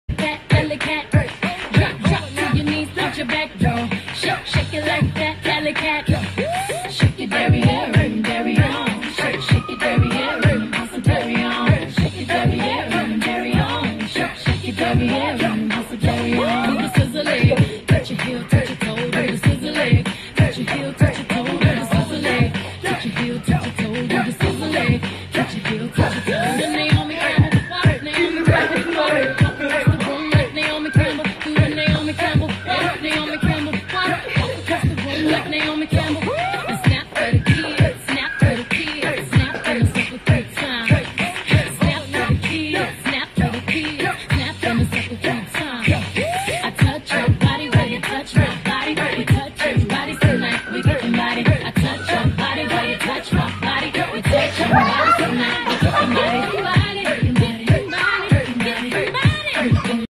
Pregnant Laura Ikeji Shows off Her Dancing Skills
Laura Ikeji is most likely the happiest pregnant woman on the planet based on her dance videos. The fashionista has been in America for months expecting to deliver her new baby any time soon. To cure boredom, she puts on music and shows the world her dance moves on social media.